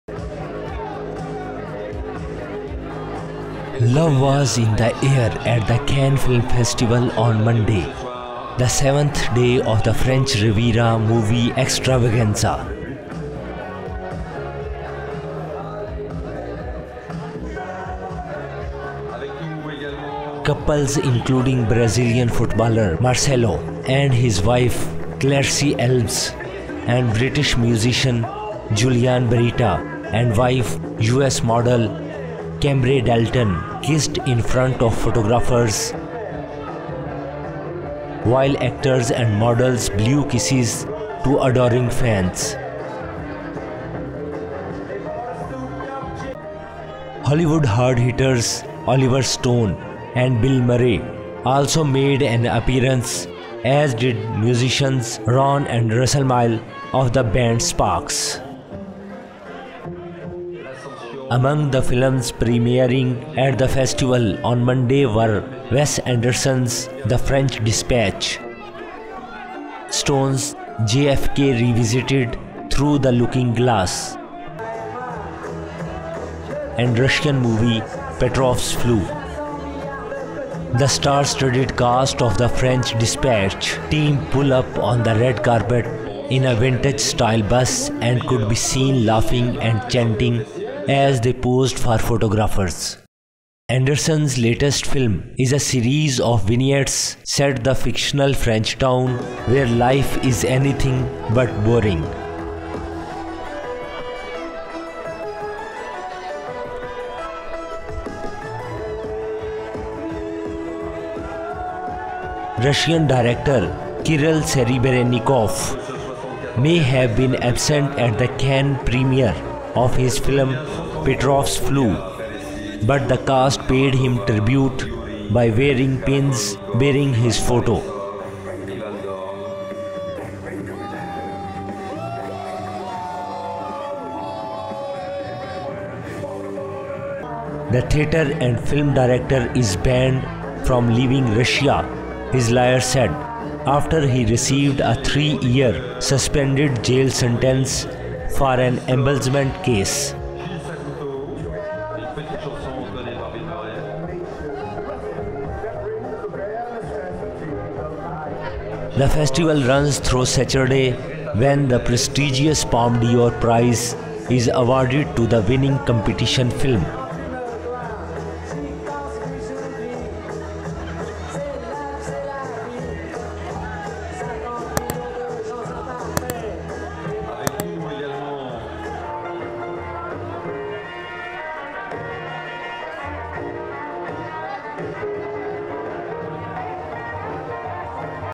Love, laughs and legends on Cannes day 7 red carpet
Love was in the air at the Cannes Film Festival on Monday (July 12), the seventh day of the French Riviera movie extravaganza.
Couples including Brazilian footballer Marcelo and his wife Clarisse Alves and British musician Julian Berretta and wife, U.S. model Kambree Dalton, kissed in front of photographers while actors and models blew kisses to adoring fans.
Hollywood hard hitters Oliver Stone and Bill Murray also made an appearance, as did musicians Ron and Russell Mael of the band Sparks.
Among the films premiering at the festival on Monday were Wes Anderson's "The French Dispatch", Stone's "JFK Revisited: Through the Looking Glass" and Russian movie "Petrov's Flu".
The star studded cast of “The French Dispatch” features Wes Anderson favourites such as Bill Murray, Owen Wilson, Tilda Swinton and Adrien Brody alongside actors Timothee Chalamet, Benicio Del Toro, Mathieu Amalric and Stephen Park.
The film team pulled up on the red carpet in a vintage style bus and could be seen laughing and chatting as they posed for photographers.
Anderson's latest film is a series of vignettes set the fictional French town of Ennui-sur-Blase - where life is anything but boring.
Russian director Kirill Serebrennikov may have been absent at the Cannes premiere of his film "Petrov's Flu" but the cast paid him tribute by wearing pins bearing his photo.
The theatre and film director is banned from leaving Russia, his lawyer said, after he received a three-year suspended jail sentence for an embezzlement case.
The festival runs through Saturday (July 17) when the prestigious Palme d'Or prize is awarded to the winning competition film.